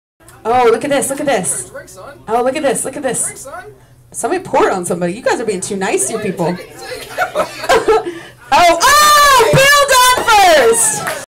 Poplar Inn - 3 Way Chug Off
How far will you go to win shots?  Do you trust your teammate?  You probably shouldn't.